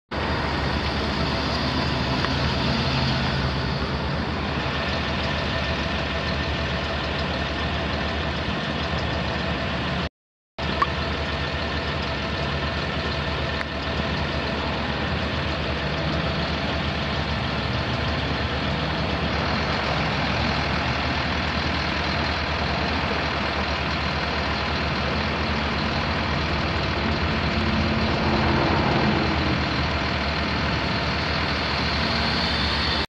2013 Ford Focus for sale at auction | bidding closes January 26, 2022
2013 Ford Focus
 Miles: 71,648 on odometer
 VIN: 1FADP3F28DL200383
Engine
 Displacement: 2.0L L4 DOHC 16V
 Cylinders: 4
 Fuel type: Gas
Transmission
 Automatic
Interior
 AC, Heat
 Cruise control
Tires
 Size: 215/50R17
Oklahoma rebuilt salvage title